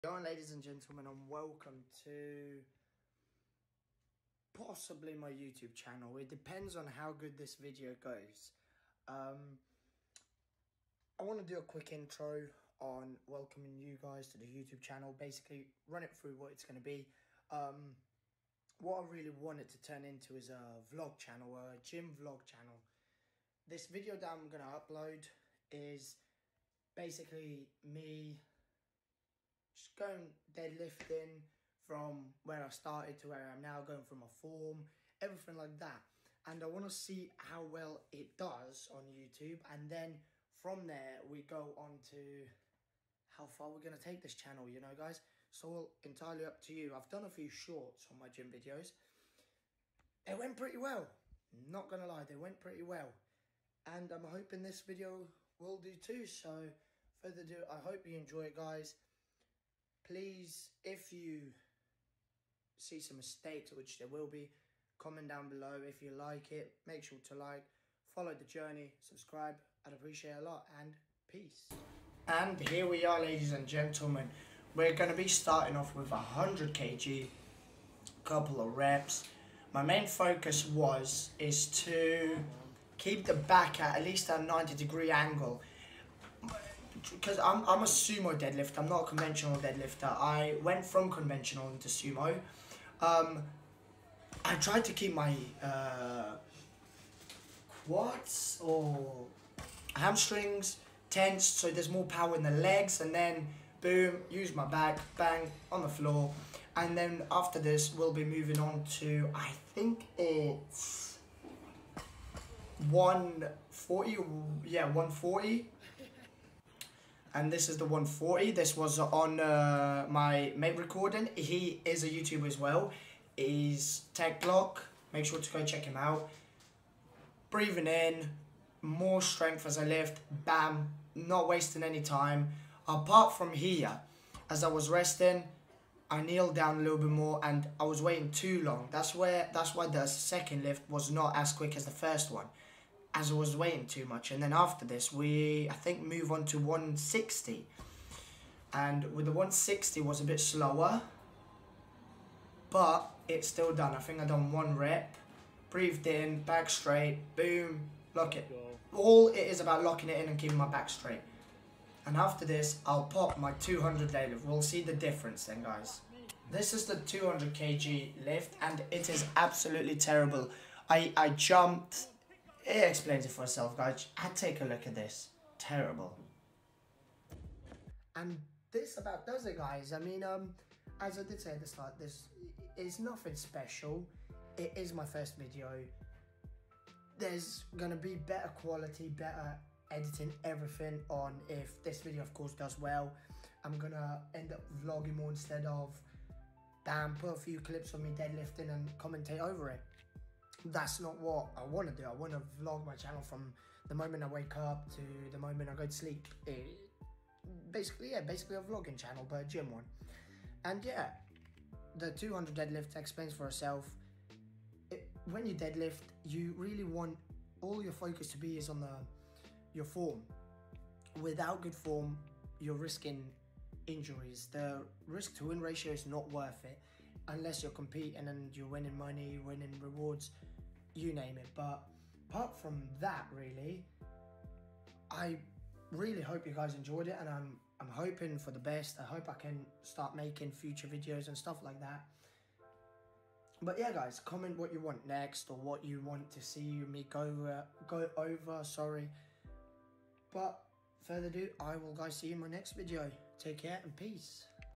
Gym Vlog--Deadlifts MAIN FOCUS FORM SPEED AND TECHNIQUE
Hey there guys, this is my first Gym video/Vlog i really hope you guys enjoy this video as much as i enjoyed making it. i hope to be making Gym vlogs a lot more, as it depends on how well this video goes as i want to see who would be interested in watching my content. HOPE YOU GUYS ENJOY